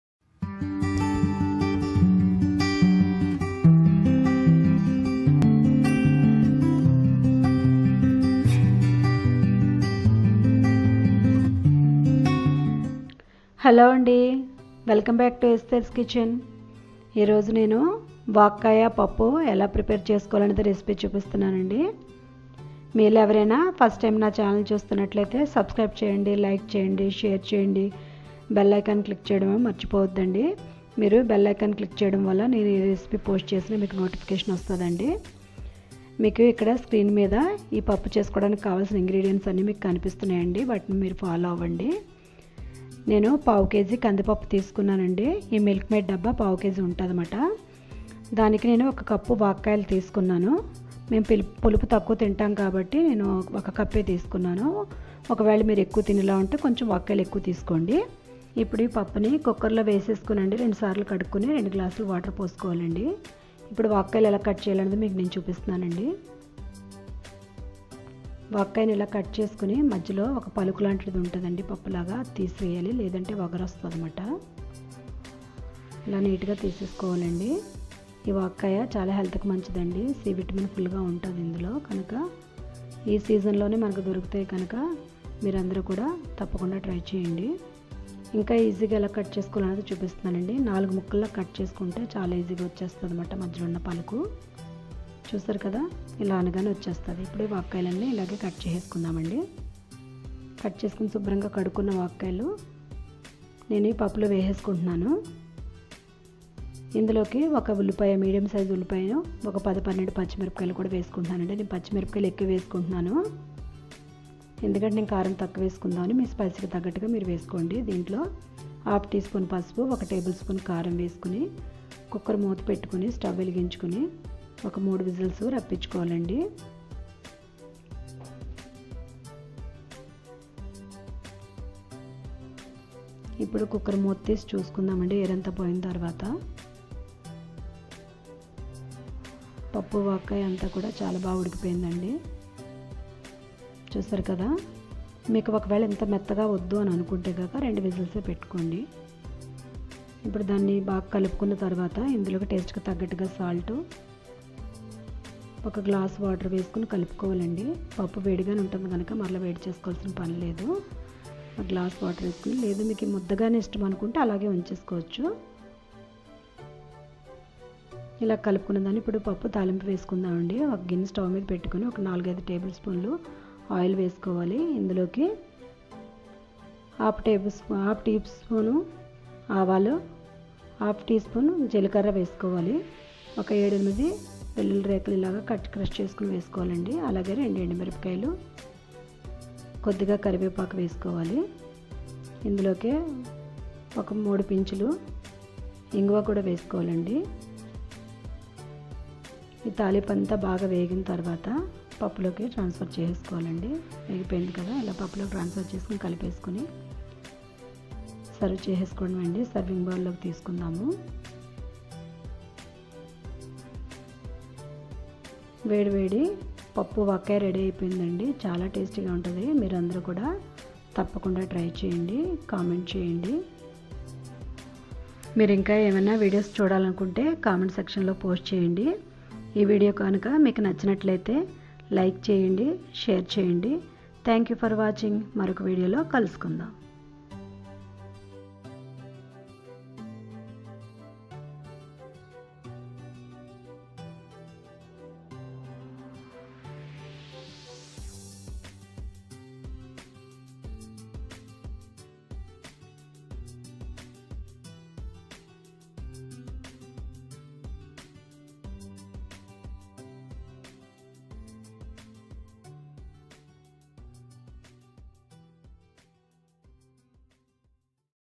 Seasonal Curry - పప్పు వాక్కాయ! || Pappu Vakkaya Recipe! || Esther's Kitchen
Hello Everyone! I am Esther, a home cook, who loves to cook delicious yet simple recipes.

Try this Pappu Vakkaya recipe and let me know how it went in the comment section below!